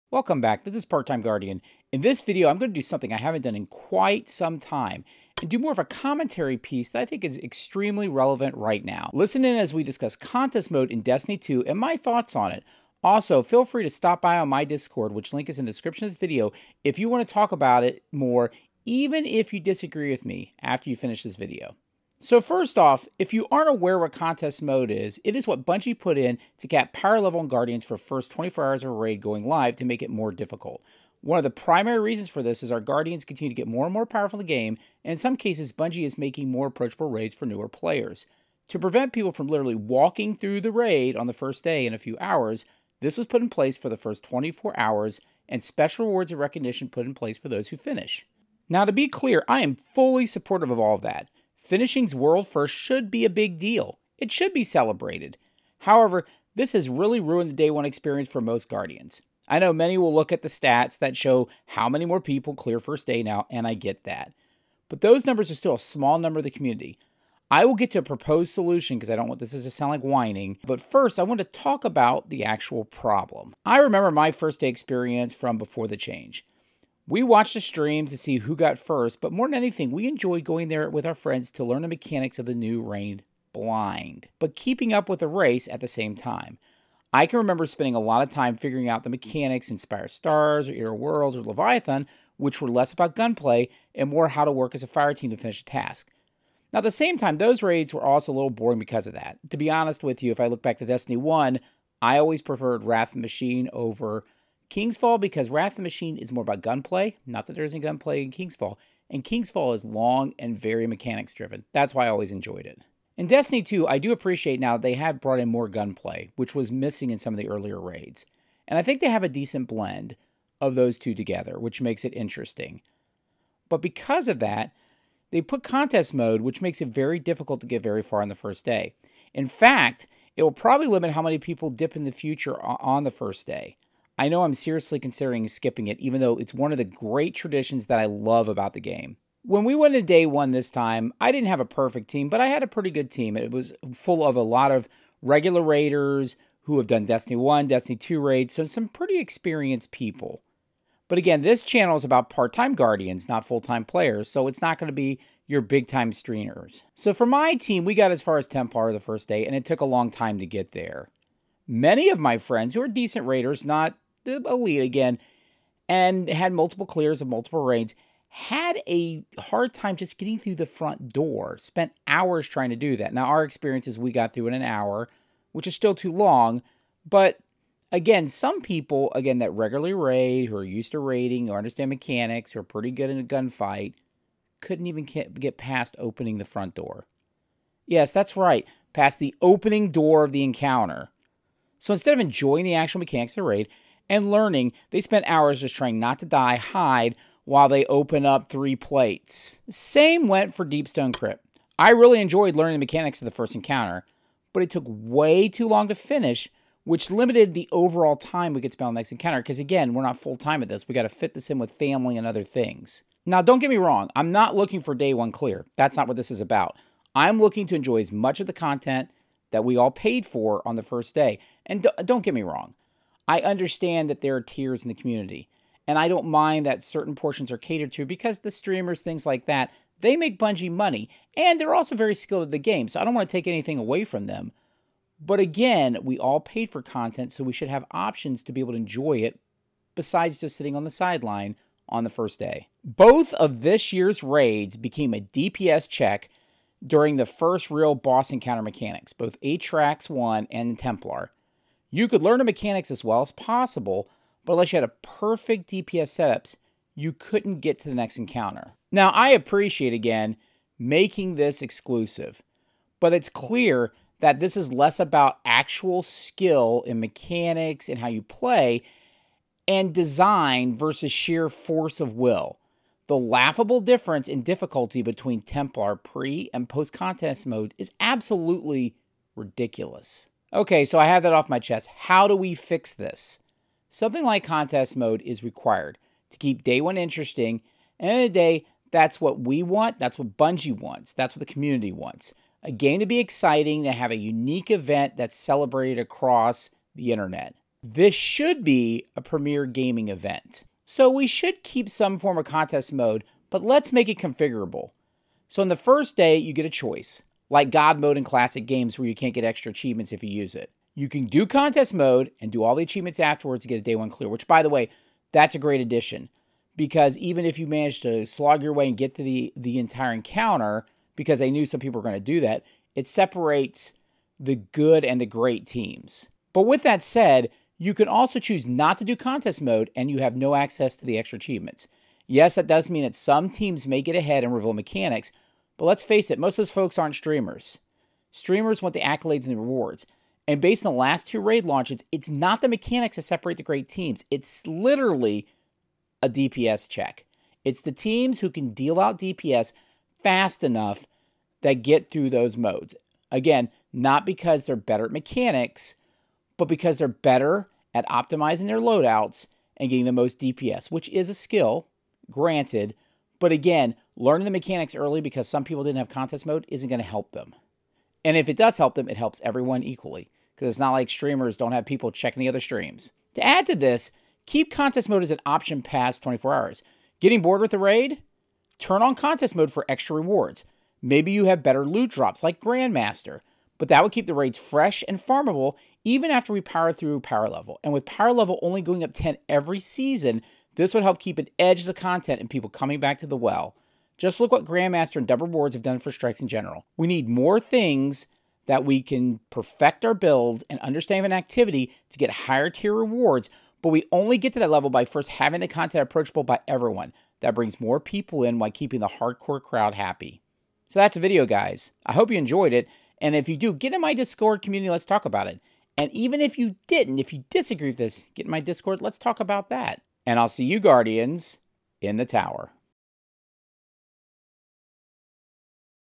Destiny 2 Rant! Contest Mode in Destiny 2 needs to change.
In this video PartTimeGuardian will tak about contest mode in Destiny 2 and how we can make it better.  Current Contest Mode which supports the worlds first raid race has some issues and I'll talk about how we fix it.  Learn about how Contest Mode was implemented for Deep Stone Crypt and Vault of Glass and the history of Day One for Raids in Destiny 1 and Destiny 2.  How can we improve the day 1 raid race experience for all guardians.  Learn more in this Destiny 2 Rant.  PartTimeGuardian has a Hot Take on Destiny 2.